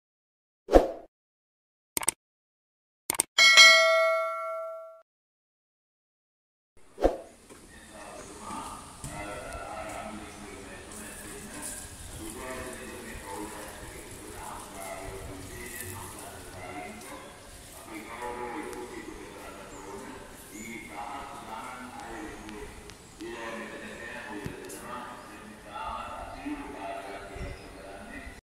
sridalada athgala wildmachan cpwildLanka slwildtv BBCNews THEWlDELEPHANT zoo tiktok animal
වසර 2009 තිරගත වූ "බිංදු" චිත්‍රපටයේ ප්‍රධාන චරිතයට පනපෙවූ  අලි පැටියා දැනට වාසය කරන්නේ ඓතිහාසික තිස්සමහාරාම රජමහා විහාරස්ථානයේ ය.මෙම හස්තියා හඳුන්වන්නේ කණ්ඩුල හස්තියා ලෙසයි...